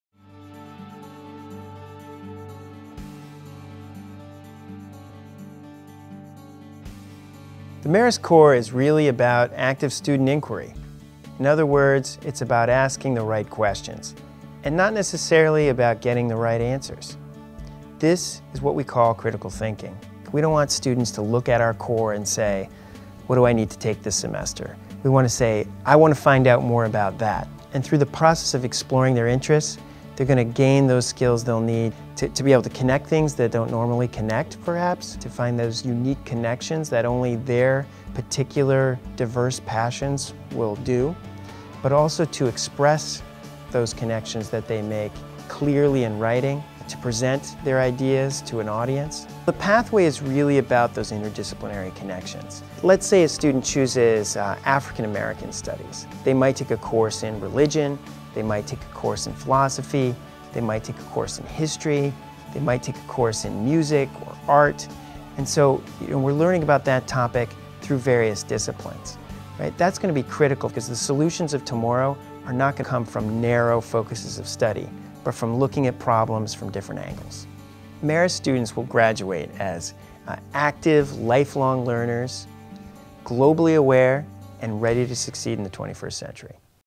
Marist: Core Curriculum
It's not just what you know. It's how you know. The Marist Core engages you in the active inquiry essential to addressing 21st-century challenges. From the First Year Seminar to the senior-level Capping course, you will integrate different areas of study, exchange ideas with faculty and your peers, and sharpen your abilities as a writer, researcher, and speaker. As indicated in the overview of the curriculum, the Marist Core offers you the opportunity to strategically shape your own educational experience.  The Marist Core fosters a strong sense of community and values awareness at the College and reflects our commitment to developing your intellect, character, and skills. We look forward to seeing what you make of it.